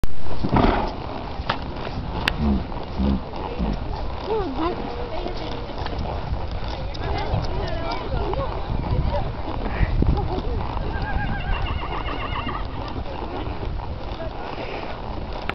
Walking around on hunter
walking hunter around after our run ,